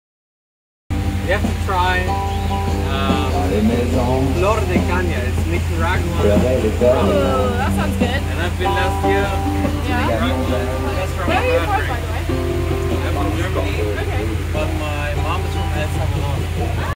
Relaxing on the slow boat from Thailand to Laos, Luang Prabang part 1
A quick video of the lovely travelers I met on the slow boat and the environment we were blessed with